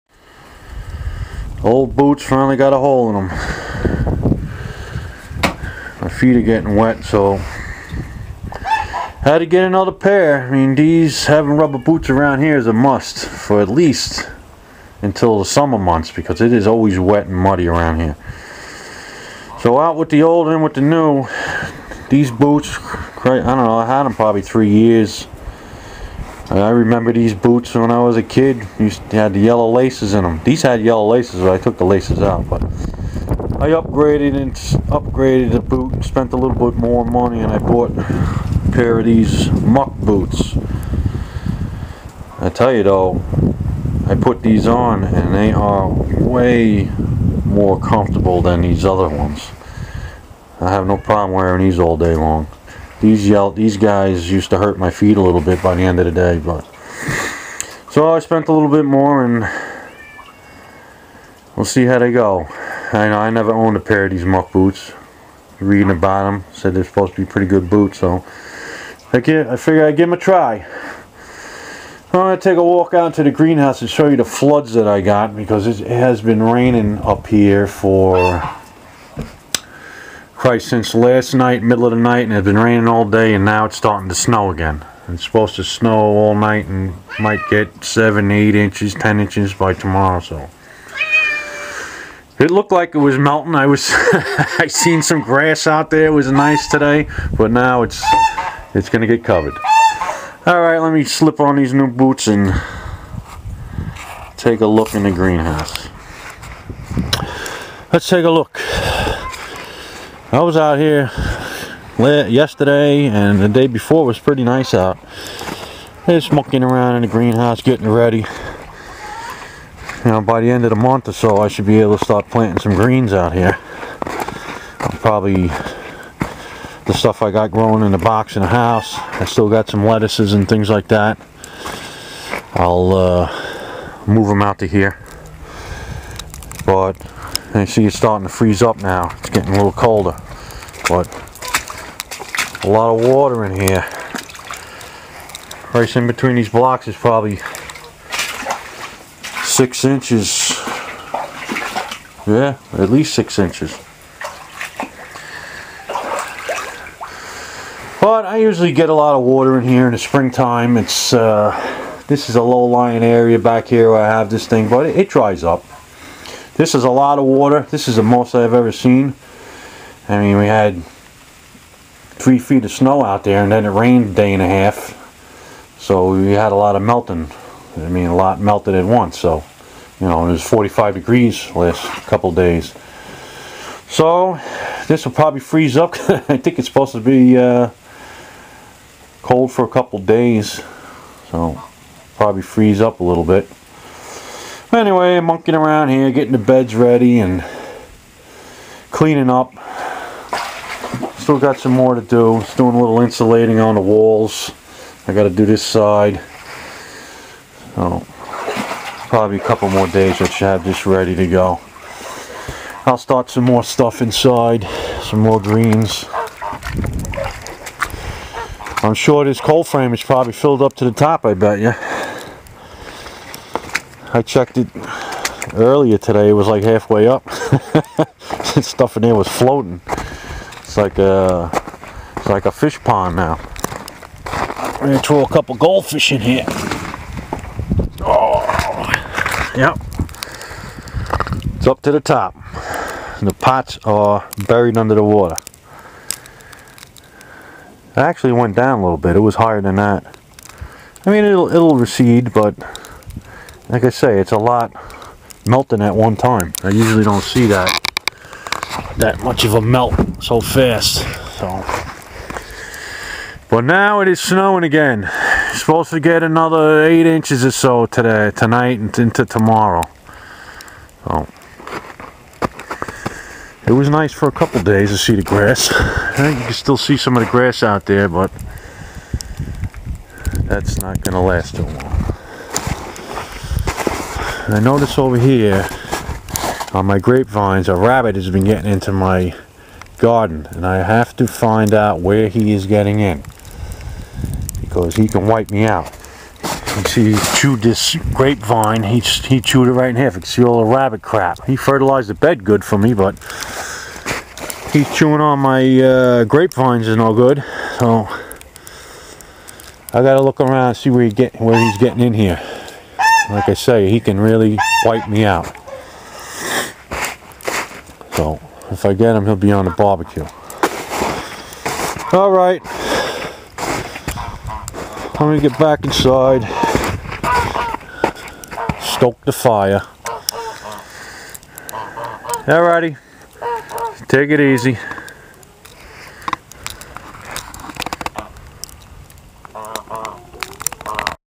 greenhouse under water
lot of rain last 24 hours